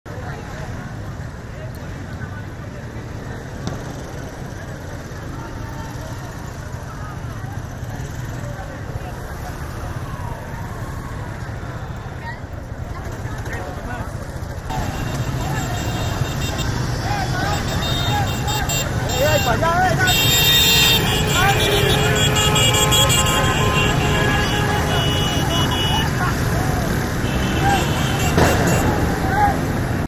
Clashes break out in Venezuela after govt political act
Clashes broke out on Friday in the Venezuelan capital after a controversial new Venezuelan assembly packed with allies of unpopular President Nicolas Maduro held its inaugural session, deepening a rift with the opposition and amid widespread international criticism of its legitimacy. IMAGES